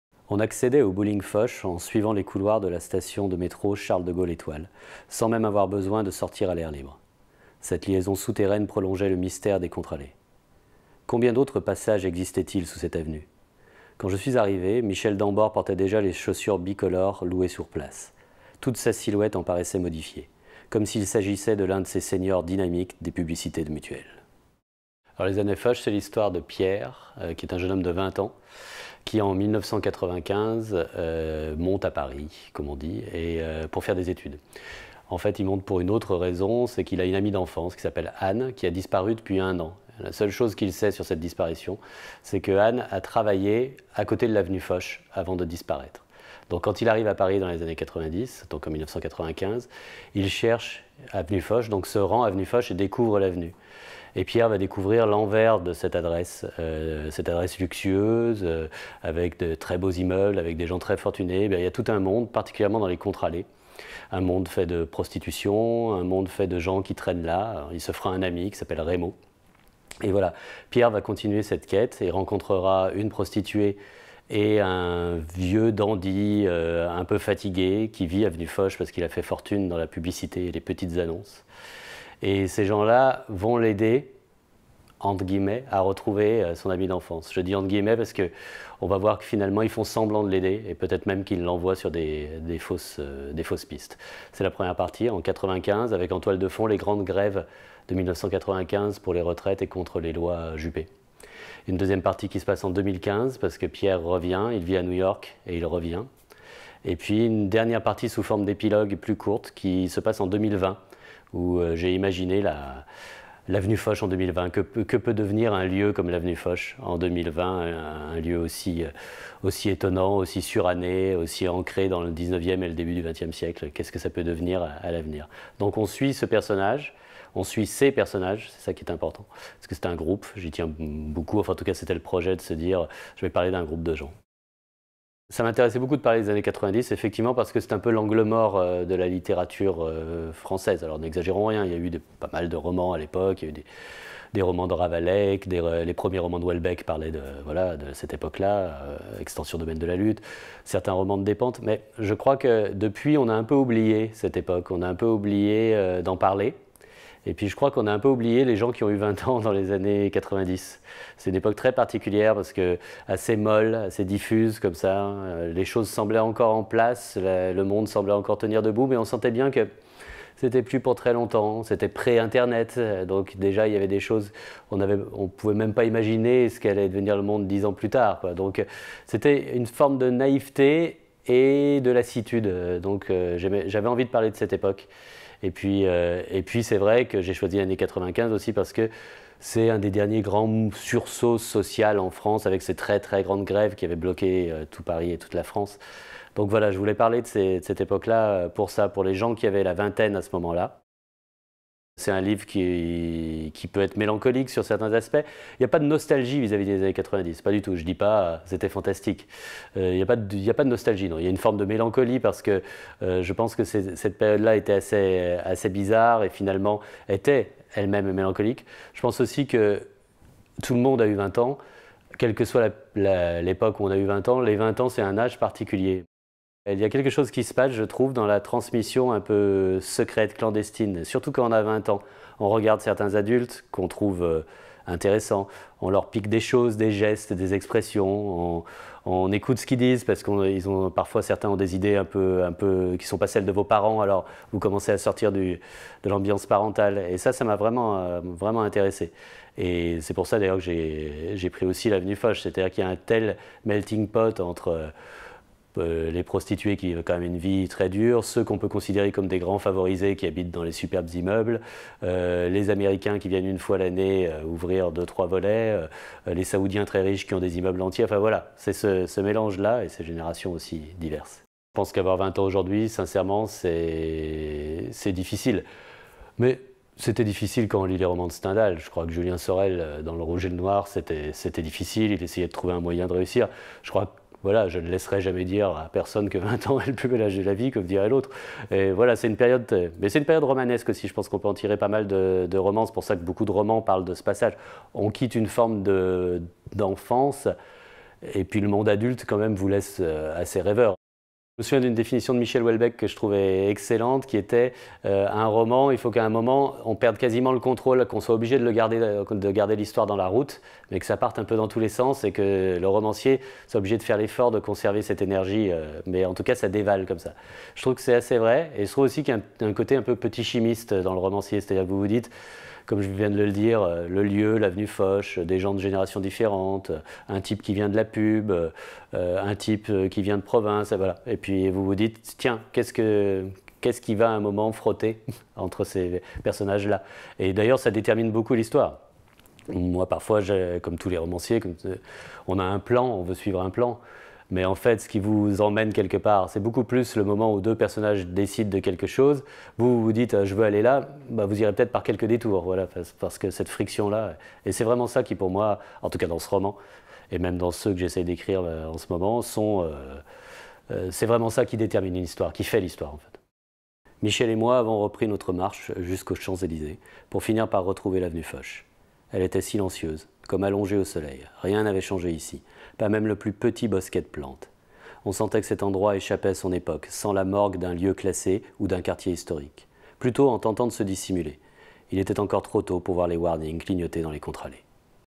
Jean-Pierre Montal - Les années Foch
Jean-Pierre Montal vous présente son ouvrage "Les années Foch" aux éditions Pierre-Guillaume de Roux. Rentrée littéraire automne 2015.